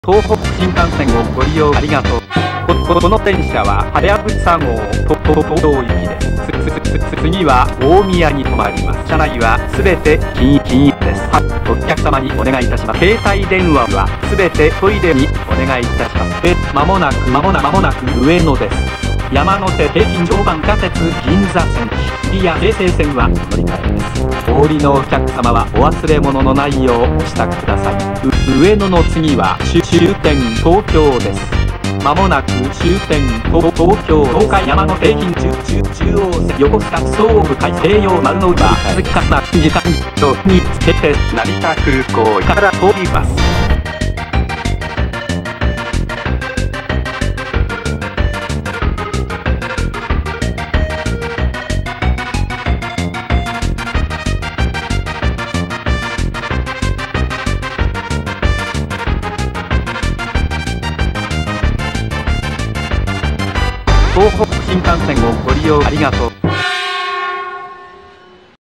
東北新幹線グルメレース
東北新幹線グルメレースです。
あさまチャンネルさんからお願いされたような気がしたので作りました！

画像一部Google
動画フリー素材
自分で撮ったのもあります。

東海道新幹線グルメレース

山陽新幹線グルメレース

北陸新幹線グルメレース